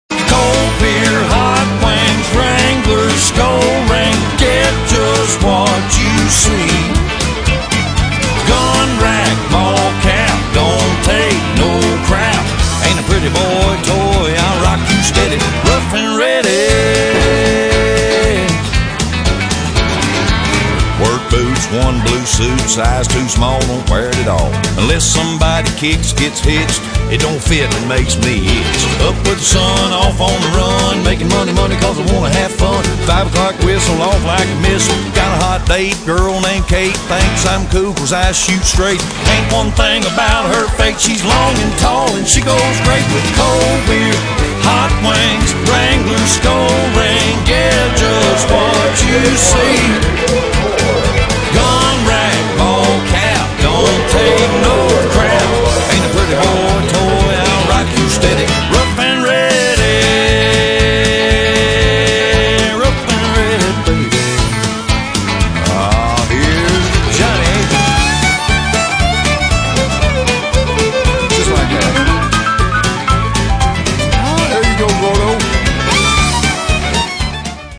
Killer & Comet down and back 4 State Youth Rodeo quarter pony
Ace & Comets first run with the 4 State Championship Youth Rodeo in Springfield, Ohio.  Ace mighta been a little tentative...but did a great job.  We expect to have a lot of fun this winter with the indoor rodeo...